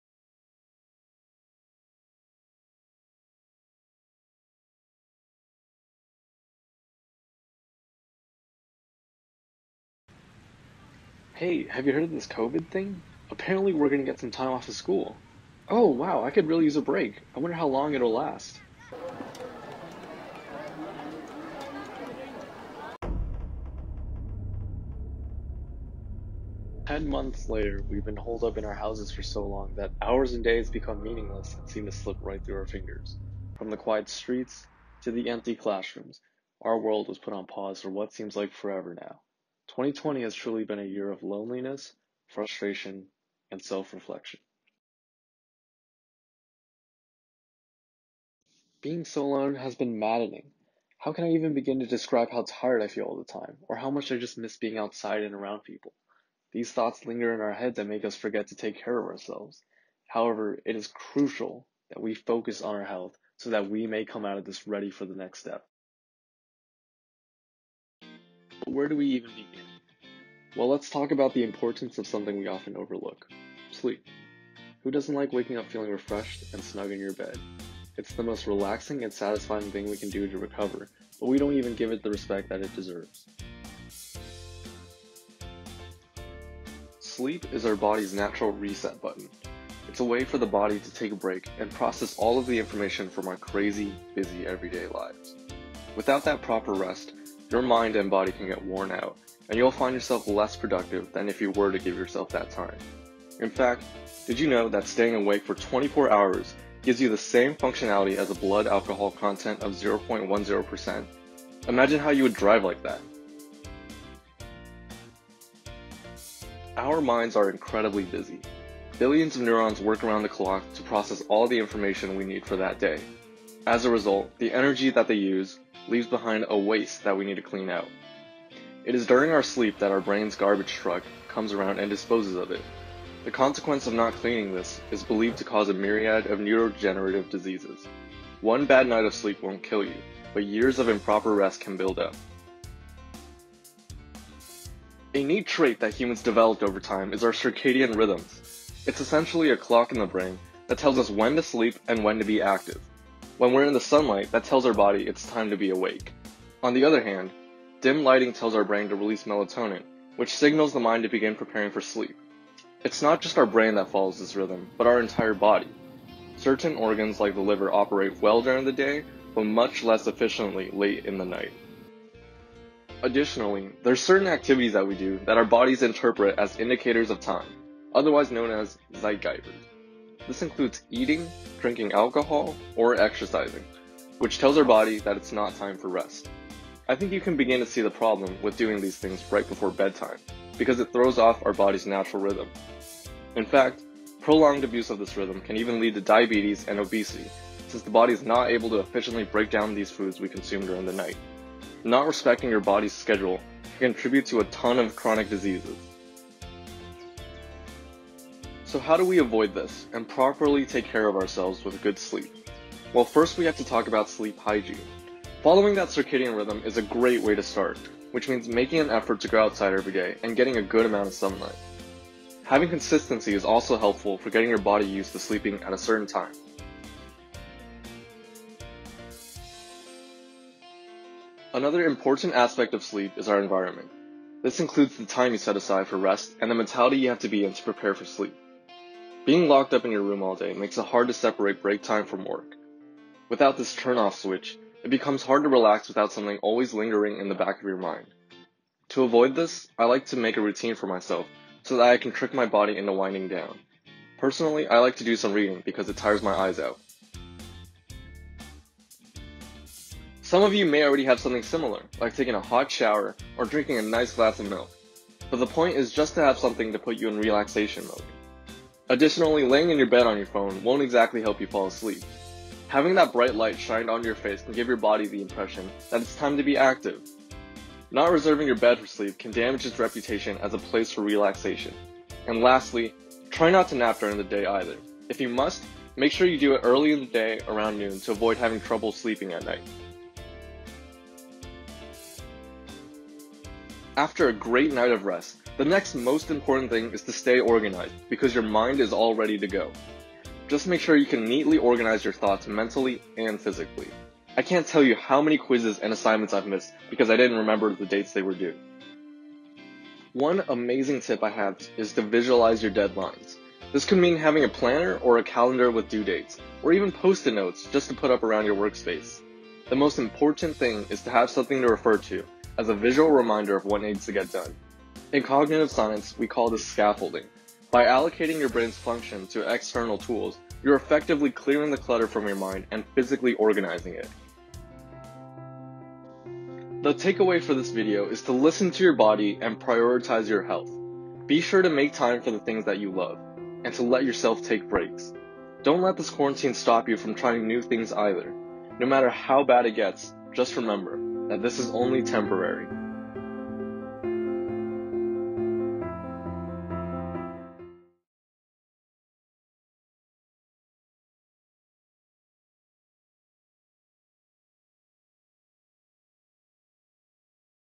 Cognitive Science Health Tips
Welcome to the Cognitive Science Student Association at UC San Diego, we are excited to meet you!

Cognitive Science can teach us a lot about how to take better care of our bodies! Whether you want to learn better health practices, organizational skills, or sleep habits, there is plenty to learn from Cognitive Science. Come learn how to live a happier healthier life!